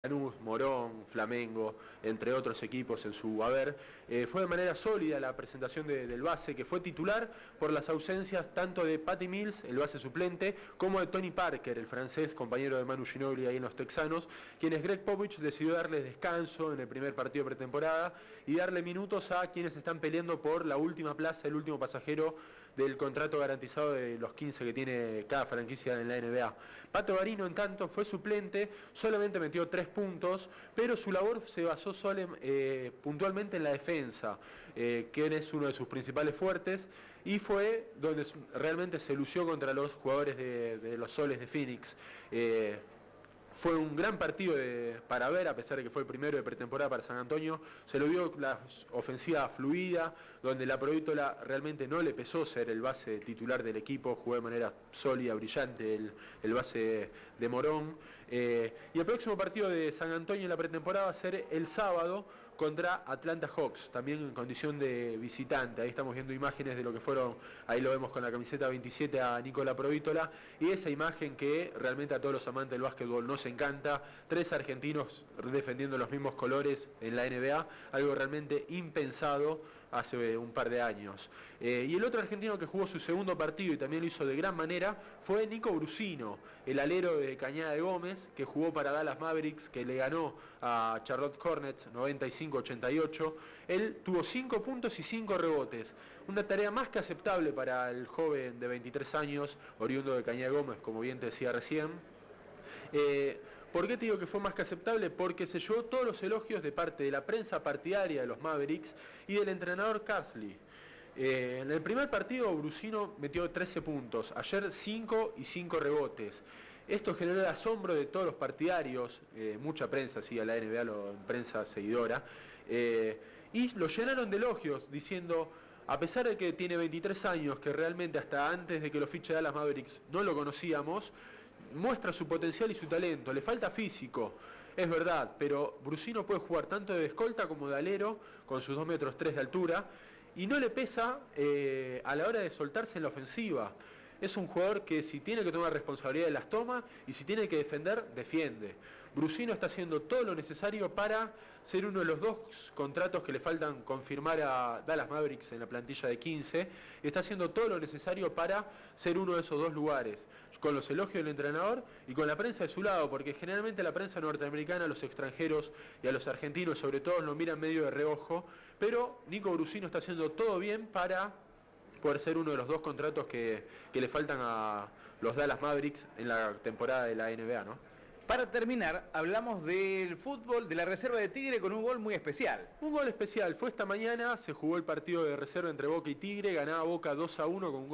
NBA: Debutaron Laprovittola y Garino en Spurs
Te lo cuentan Gustavo Cappucio y Mariano Attanasof.